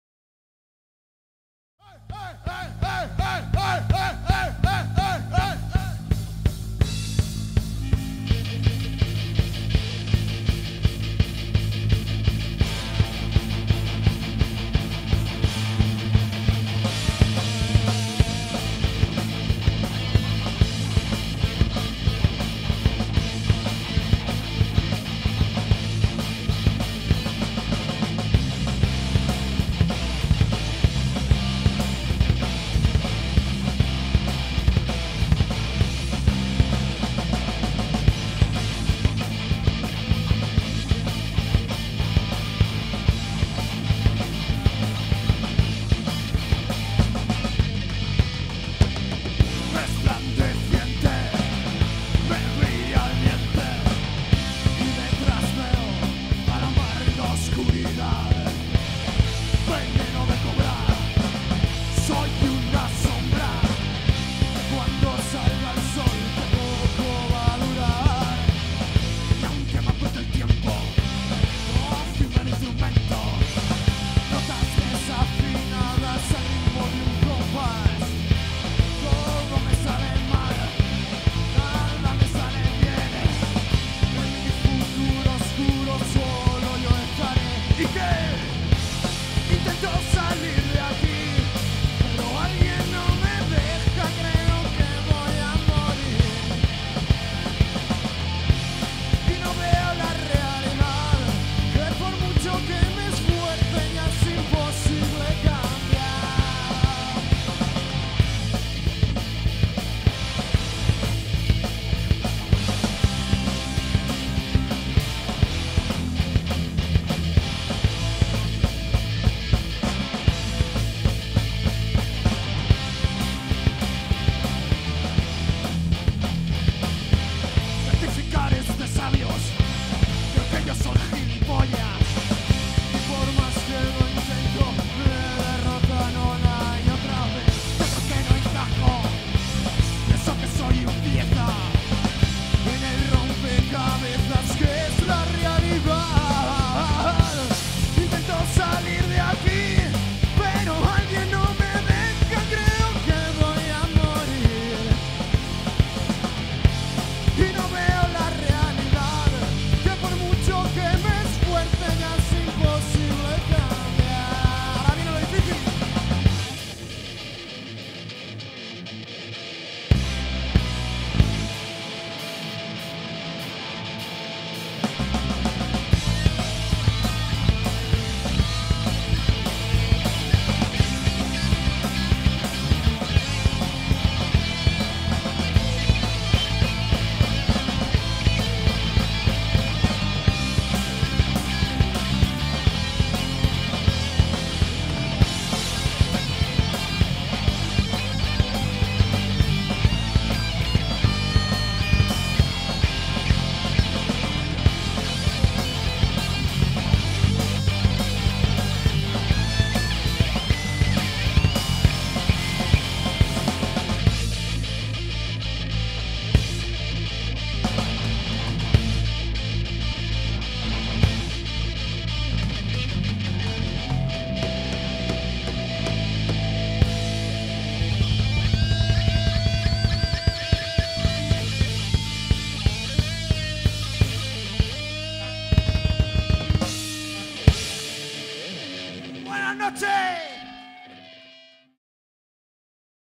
Mas Ke Rock - II Festival Más Ke Rock 4/5
Programa musical del canal CBT de Cabeza del Buey. Reportaje del II Festival Más Ke Rock, actuación de Dsgarre. 4ª Parte.